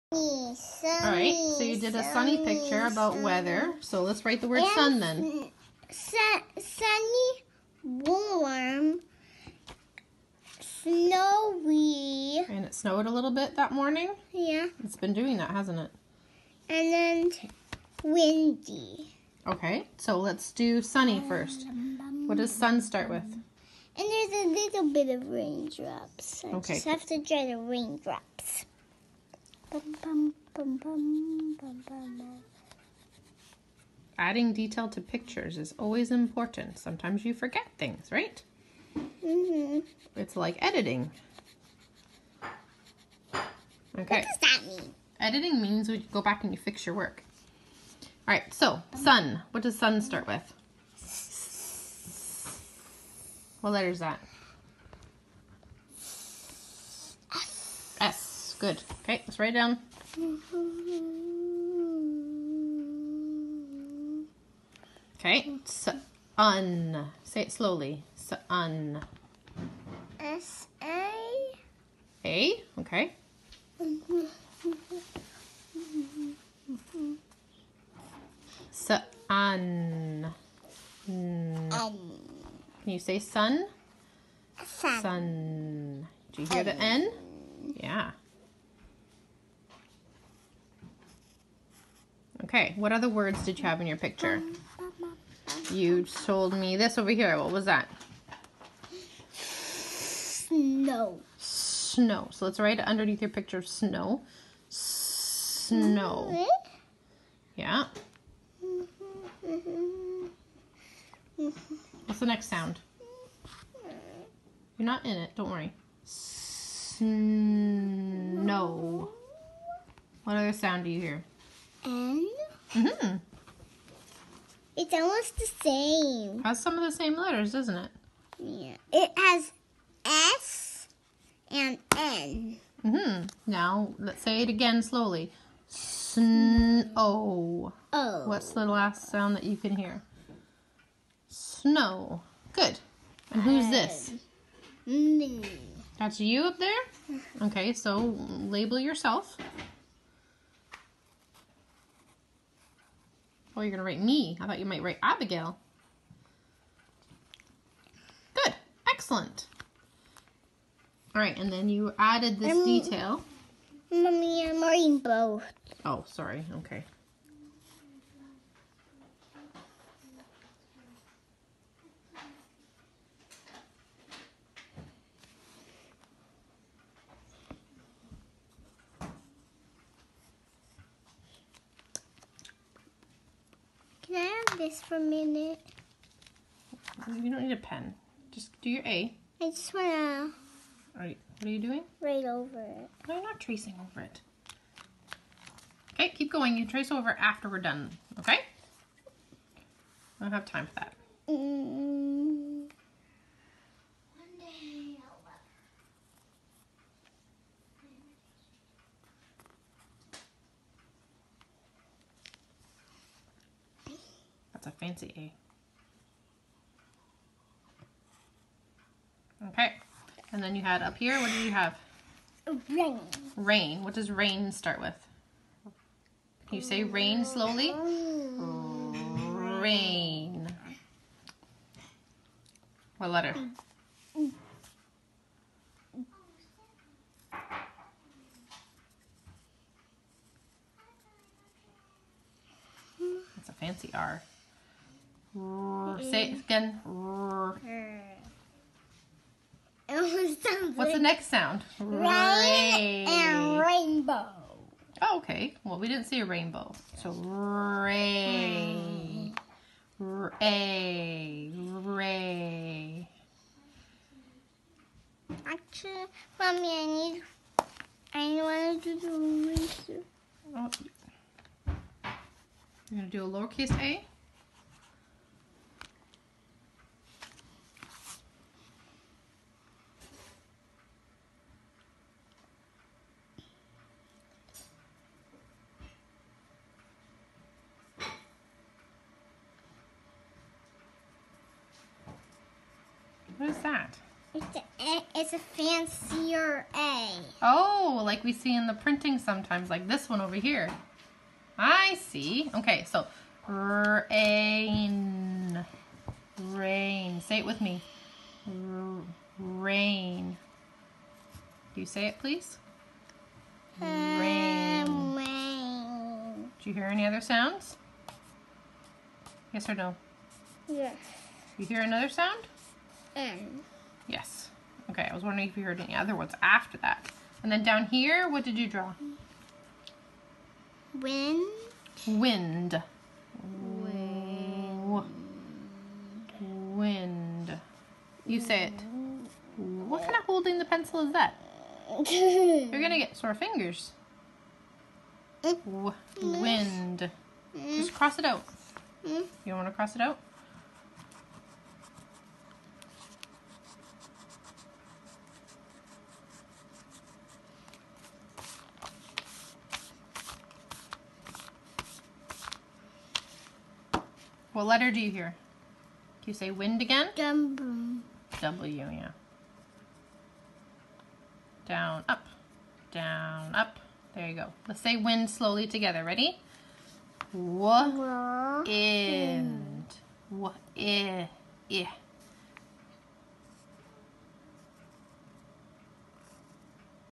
April 20-25: Part 1 of 2 - Modeling Writers Workshop
Here my daughter is labeling her picture and making a word bank.  She drew a picture of the weather she saw outside.